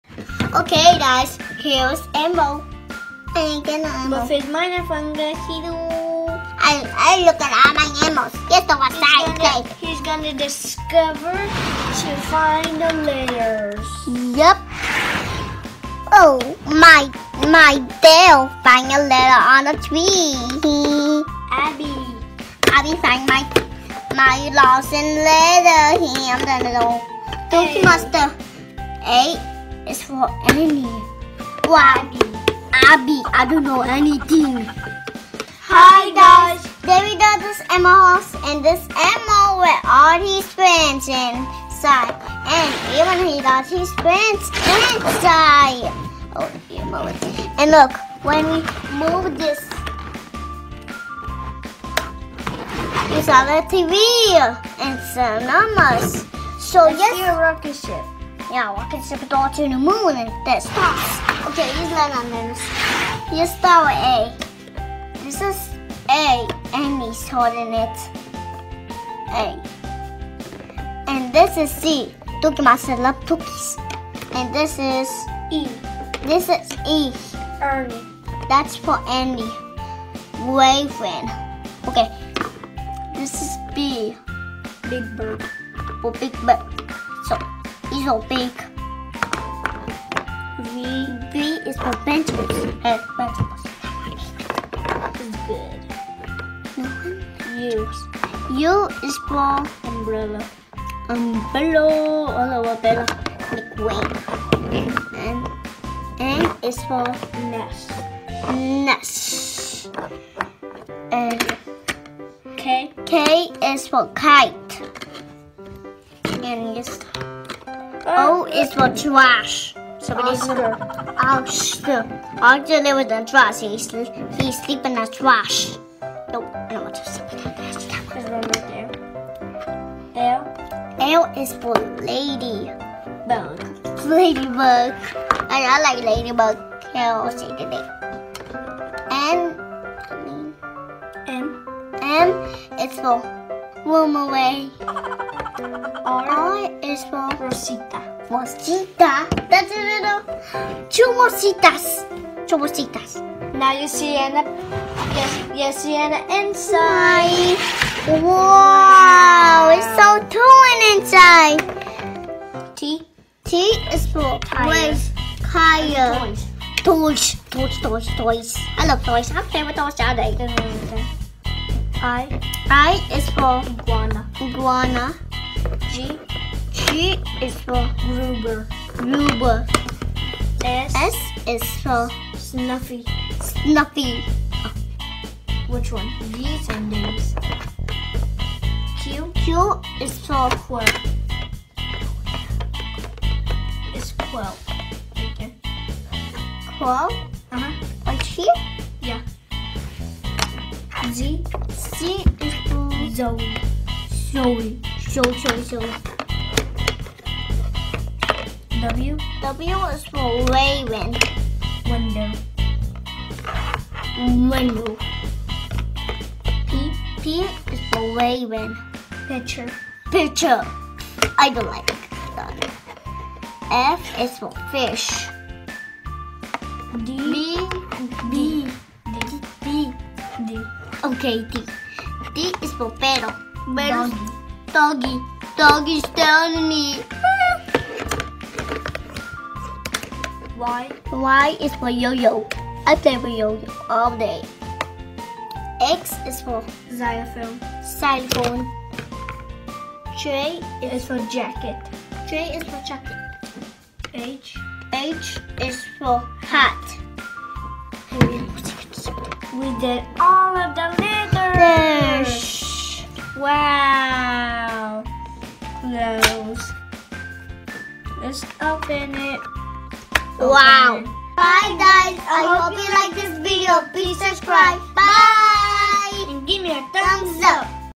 Discover ABCs with Elmo Playset | Lupita Learning Her ABCs with Leo ABCDEFGHIJKLMNOPQRSTUVWXYZ
Hello friends!
We just got this awesome playset to learn our ABCs!
Discover ABCs with Elmo Playset!

A - is for Abby
B - is for Big Bird
C - is for Cookie Monster
D - is for Dog
E - is for Ernie
F - is for Fish
G - is for Grover
H - is for Hat
I - is for Iguana
J - is for Jacket
K - is for Kite
L - is for Lady Bug
M - is for Murray
N - is for Nest
O - is for Oscar
P - is for Picture
Q - is for Quilt
R - is for Rosita
S - is for Snuffy
T - is for Toys
U - is for Umbrella
V - is for Vegetables
W - is for Window - Wave
Z - is for Zoe